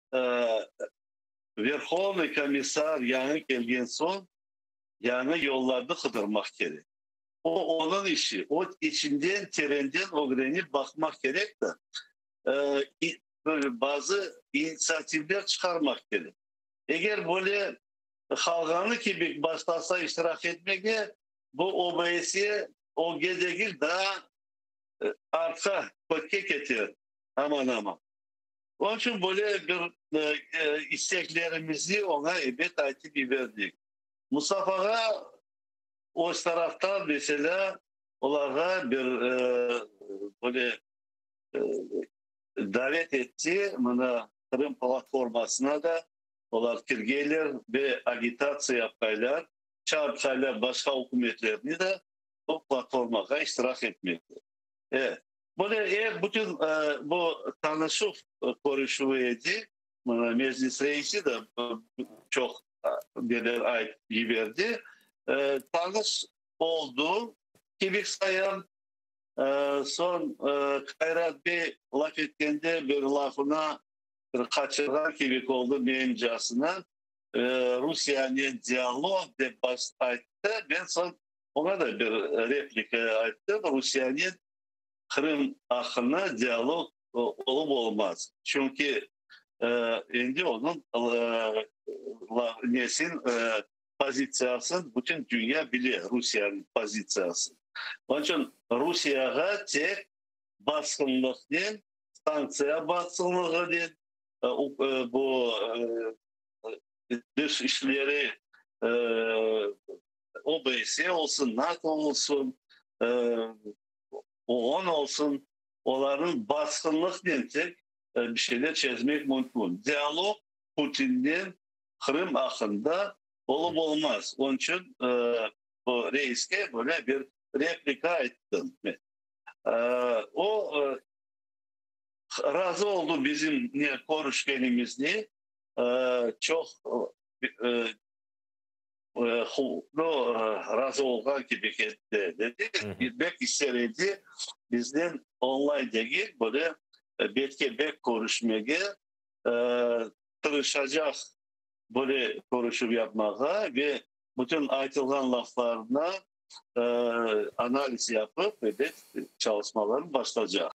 RUSİYE İLE QIRIM BOYUNCA DİALOG OLMAYCAQ: MECLİS OSCE KOMİSSARINA
Карта Монобанк:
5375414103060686
Карта ПриватБанк:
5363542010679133

Діалогу по Криму з Росією не буде: Меджліс комісару ОБСЄ

Під час зустрічі Меджлісу кримськотатарського народу з новим комісаром ОБСЄ у справах національних меншин Кайратом Абдурахмановим, члени представницького органу кримських татар наголосили на неможливості діалогу з владою РФ щодо окупованого півострову.

Які пропозиції висунув Меджліс, та яку допомогу ОБСЄ може надати у кримському питанні?

Коментує Різа Шевкієв — член Меджлісу кримськотатарського народу